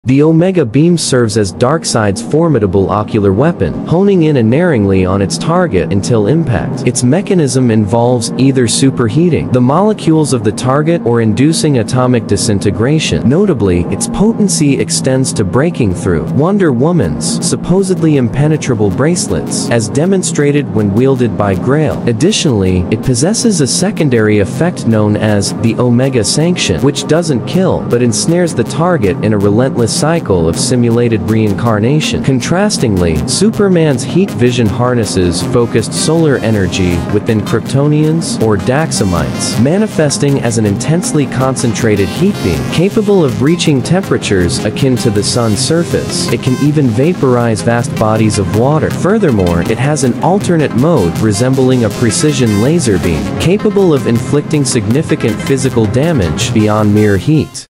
The Difference Between Omega Beam vs Heat Vision
This is where things get lopsided. Omega Beams are capable of destroying entire planets. Heat Vision, while powerful, is typically used to level buildings or overpower enemies